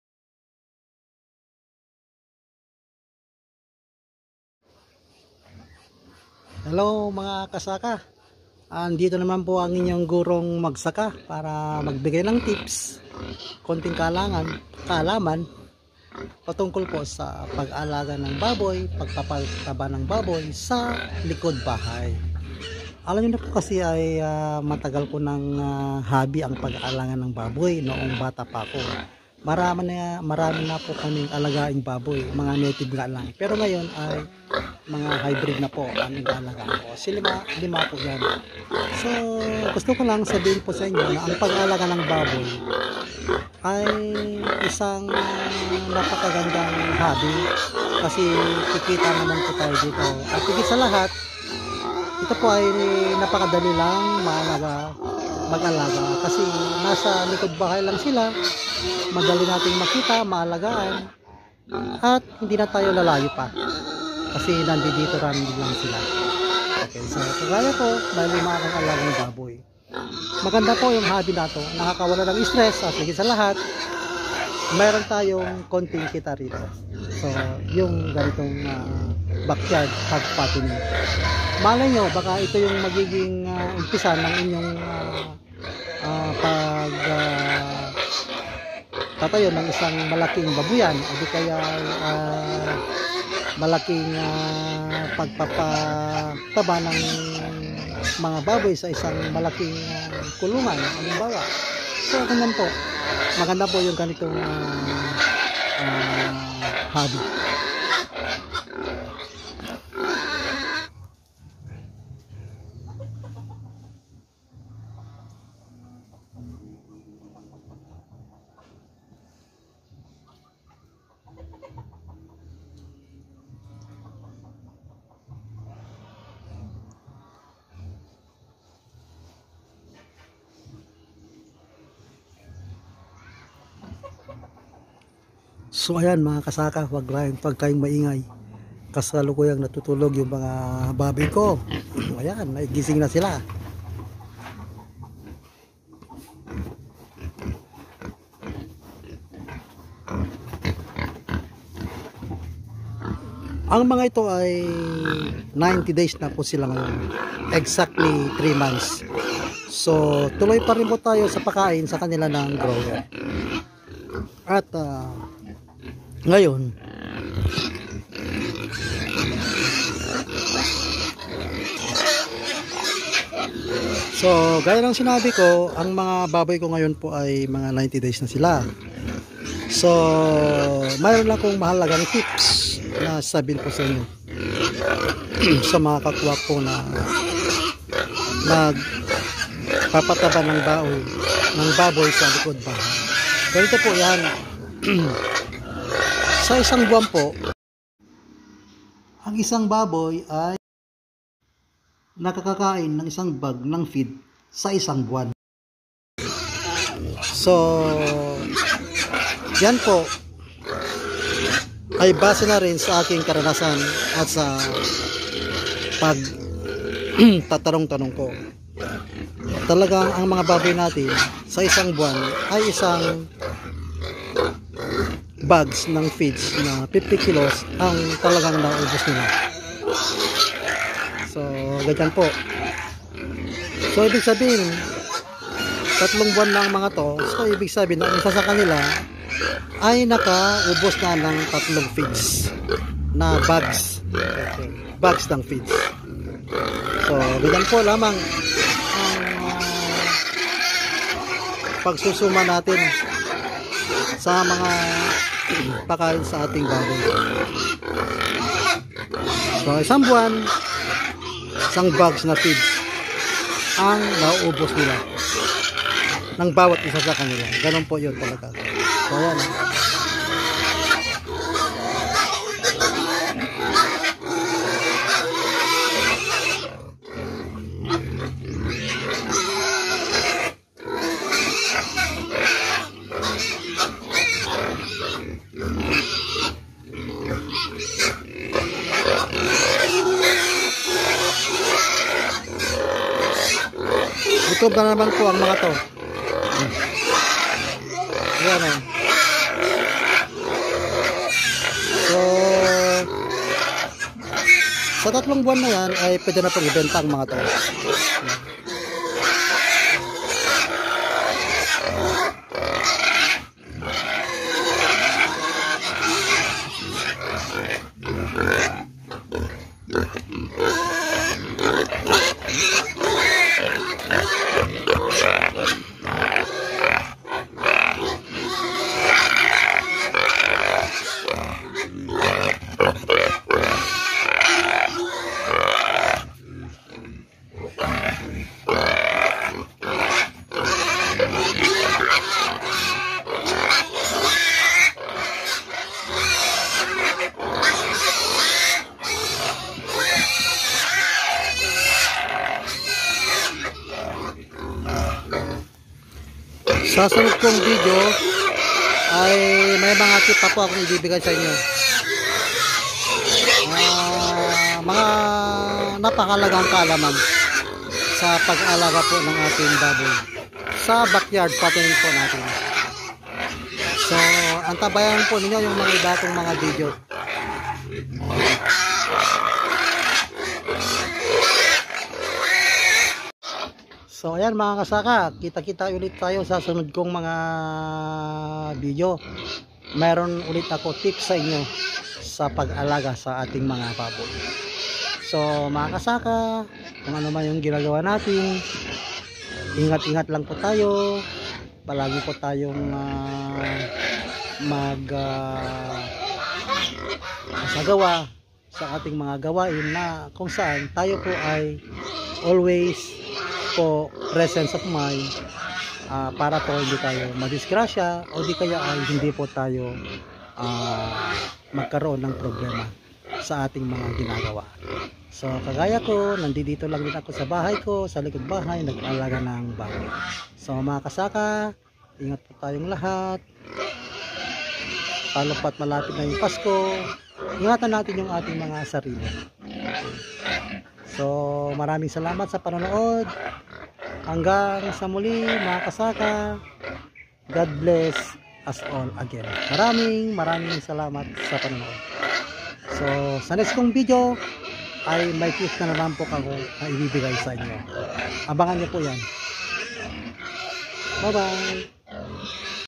1 BABOY ||1 SAKO NG FEEDs || 1 BUWAN?
Mga kasaka ang isang patabaing babpy ay nakakaubos ng 1 sakp ng feeds sa isang buwan at ibinabahagi ko ito sa inyo para magkaroon kayl ng idea sa gastos sa pakain sa mga patabaing nating baboy salamat kasaka God bless!